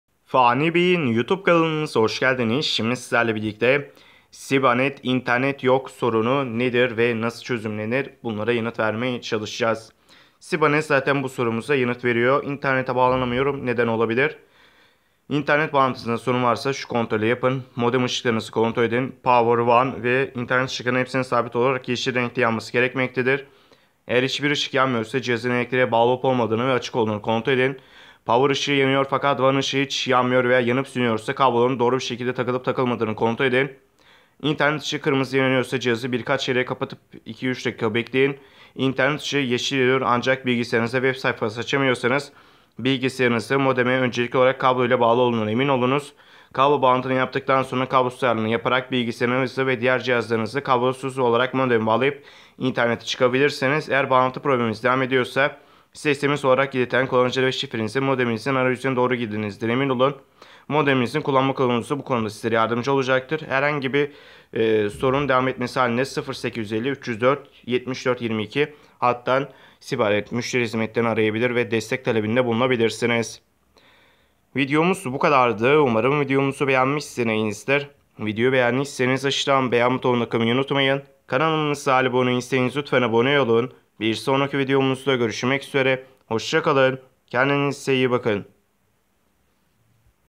Sibanet İnternet Yok Ne Demek?
Merhabalar bu videomuzda sizlerle birlikte sibanet internet yok sorunu çözümü nasıl yapılır sorusuna yanıt vermeye çalışıyoruz.İyi Seyirler Dilerim.

sibanet internete bağlanamıyorum
sibanet internet yok hatası çözümü
sibanet internet yok ne demek